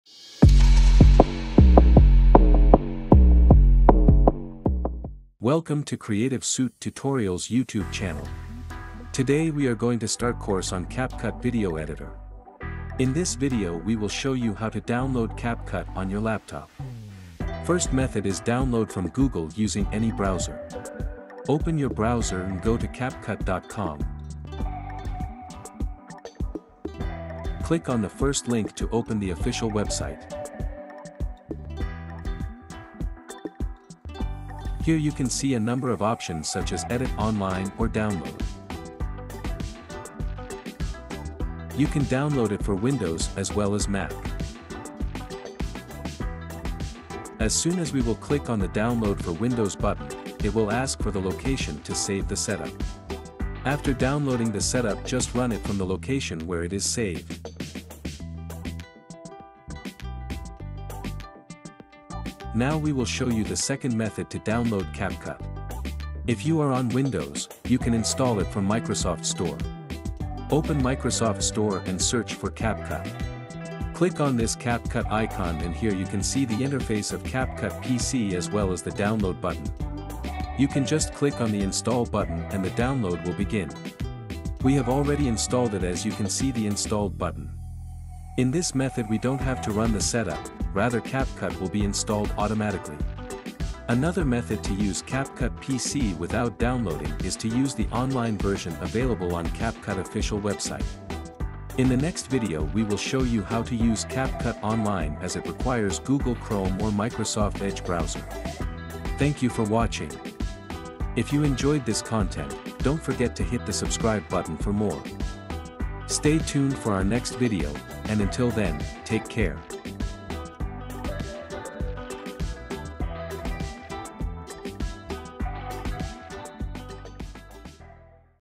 How To Download Capcut On Laptop Without Bluestacks | Capcut Video Editor | Capcut Tutorial 2023
Discover the power of video editing with CapCut, the ultimate tool for creating stunning content. In this tutorial, we'll show you two methods for downloading CapCut on your PC. Whether you're using Windows or Mac, you'll learn how to easily install this powerful video editing tool and start creating amazing content. With CapCut, you'll have all the tools you need to edit like a pro, including advanced features and a user-friendly interface. This video will guide you through each step of the download process, so you can start editing right away. By the end of this tutorial, you'll have the knowledge to create captivating videos with CapCut video editing. So why wait? Download CapCut now and start creating amazing content with our CapCut edit tutorial.